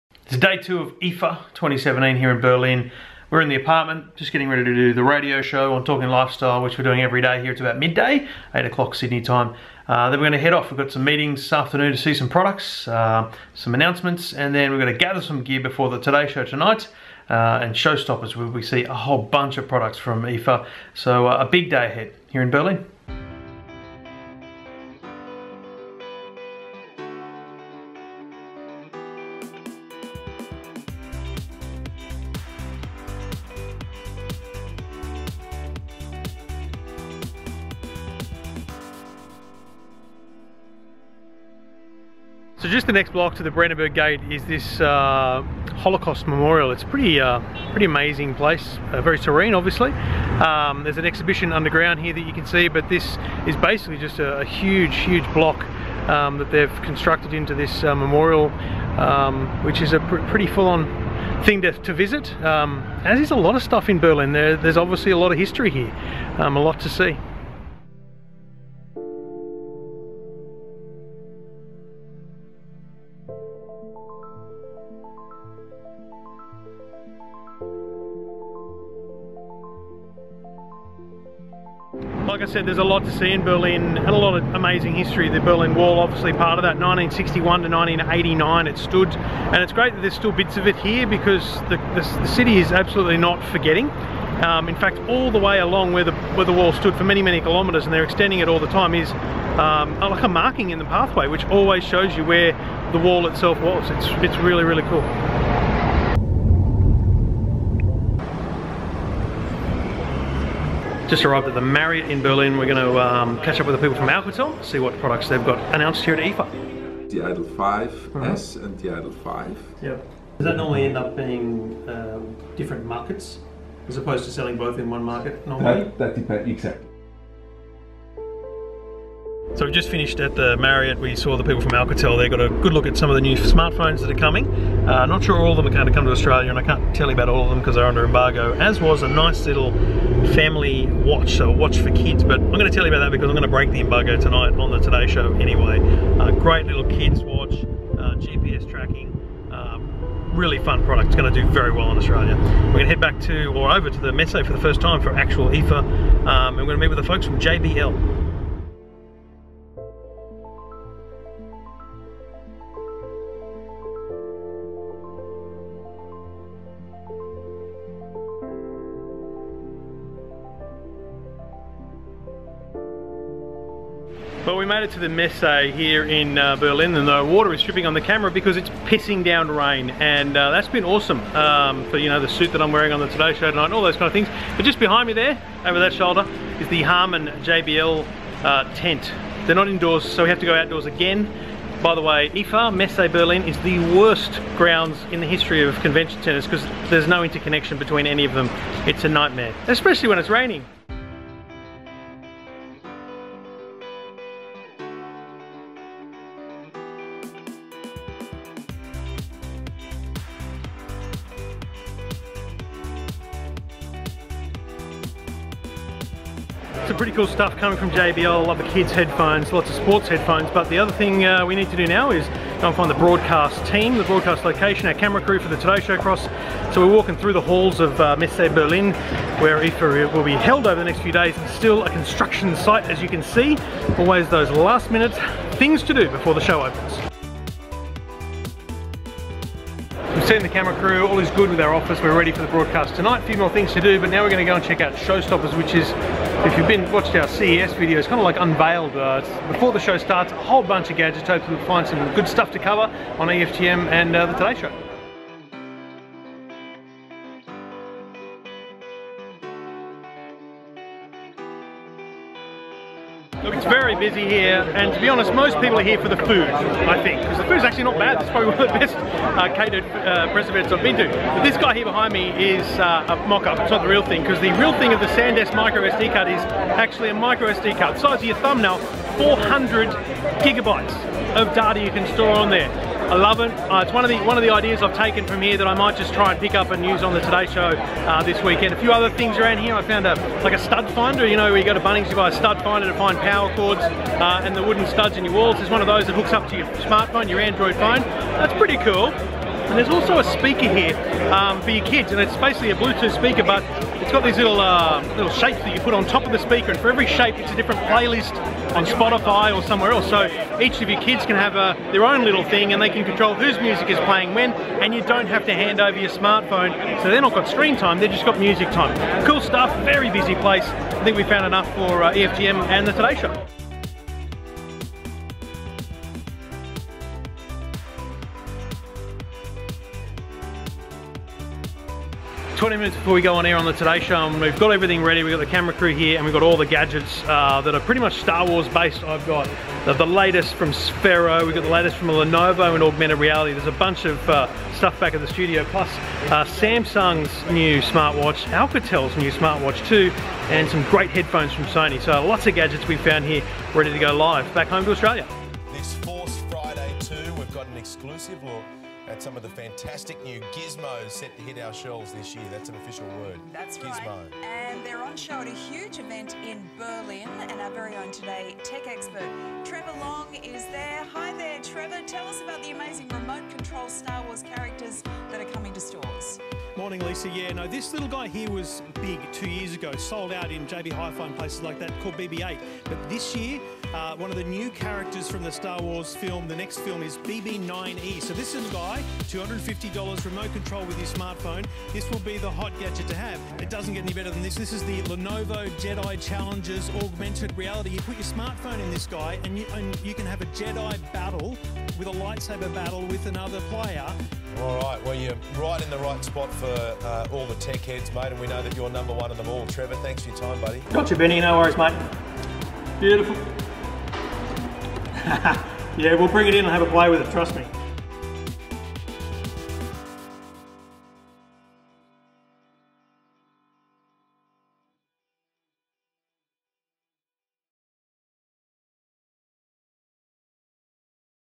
Berlin Wall, Showstoppers, Speakers & STAR WARS - IFA Vlog 2 thanks to Vodafone.
The Berlin Wall, Showstoppers, watery JBL speakers & Star Wars.